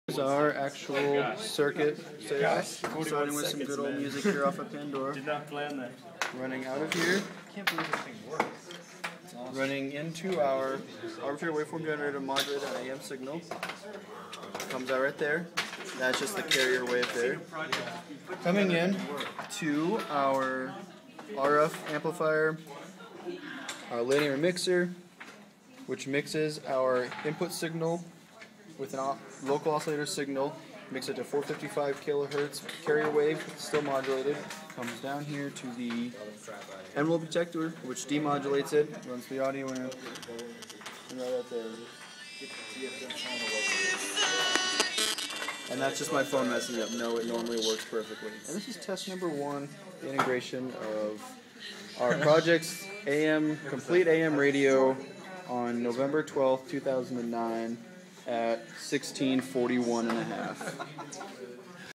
Electronics I Lab AM Receiver Project
This was an AM receiver built by a team of 5 Junior engineering students for the Electronics I Lab section at LeTourneau University.  It receives on 550KHz and goes through an RF filter and amplifier, into a mixer with a local oscillator to mix it to the heterodyne frequency of 455KHz then passes through the envelope detector (demodulator) and then into the audio amplifier and to the speaker.  It is being fed a signal from a computer fed through an AM modulator/arbitrary waveform generator since no local stations broadcast at 550KHz.